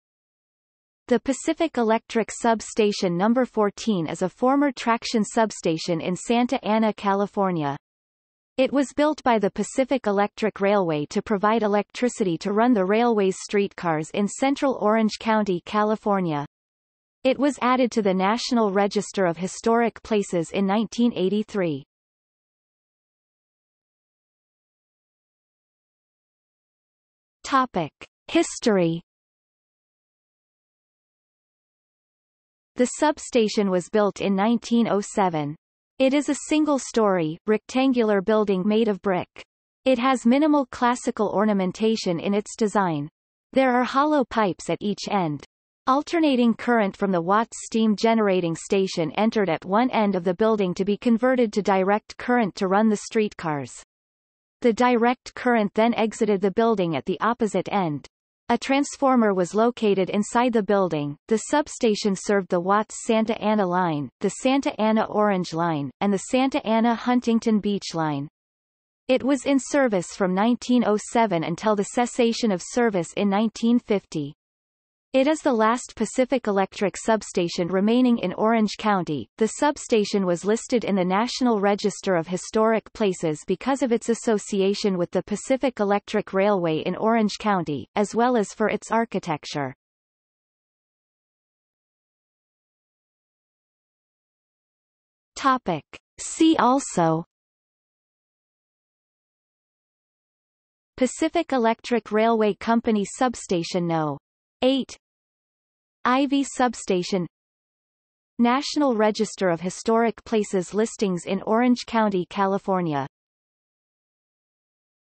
Pacific Electric Sub-Station No. 14 | Wikipedia audio article
00:00:23 History
00:01:31 See also

- Socrates

SUMMARY
The Pacific Electric Sub-Station No. 14 is a former traction substation in Santa Ana, California. It was built by the Pacific Electric Railway to provide electricity to run the railway's streetcars in central Orange County, California.  It was added to the National Register of Historic Places in 1983.